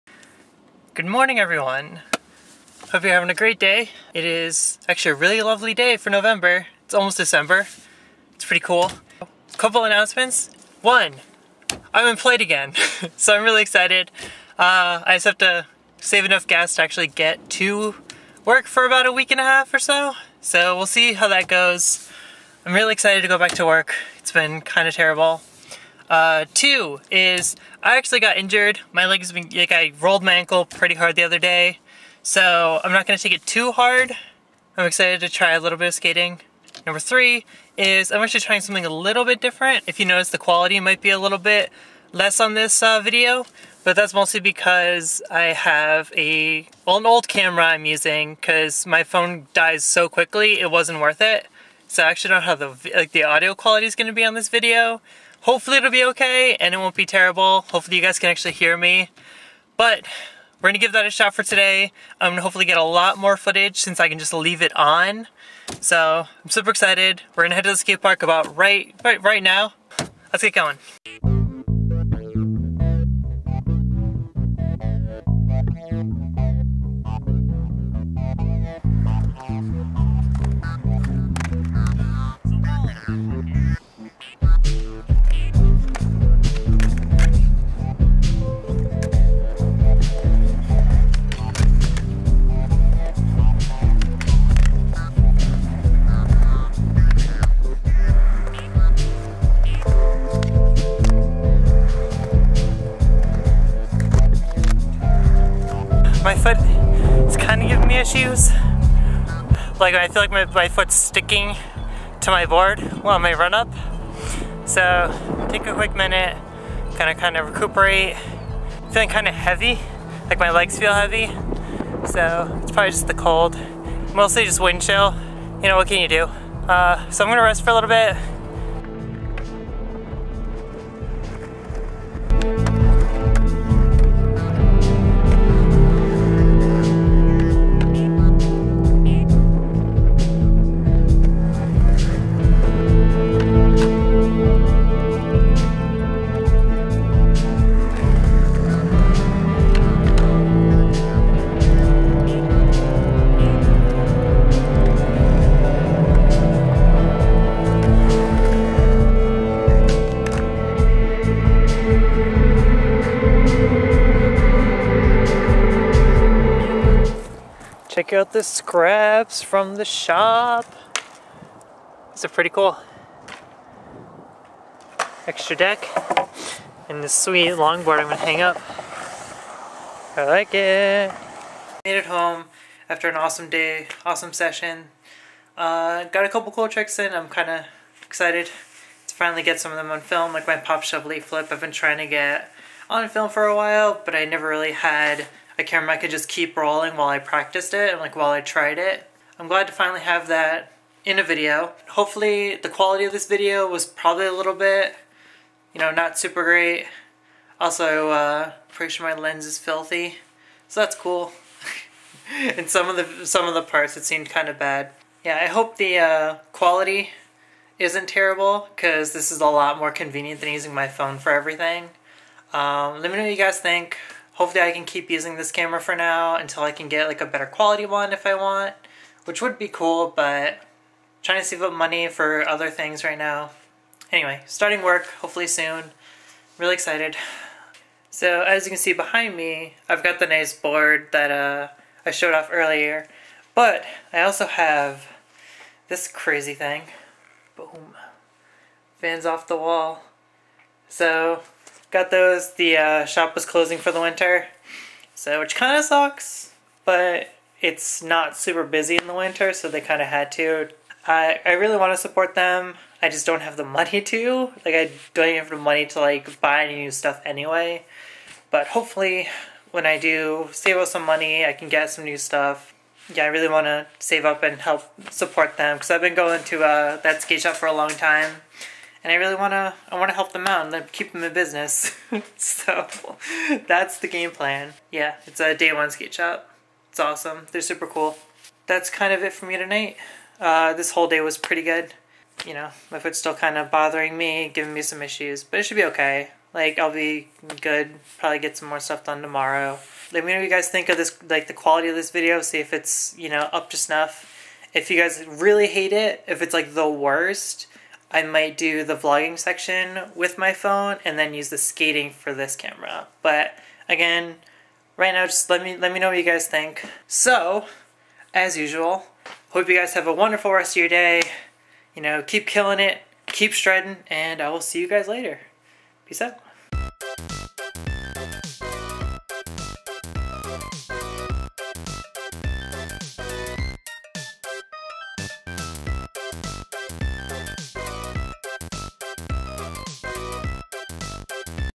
NEW (old) camera! Vlog #6
Long time since I did a vlog. Kinda been a rough week. Things should be looking up from now though.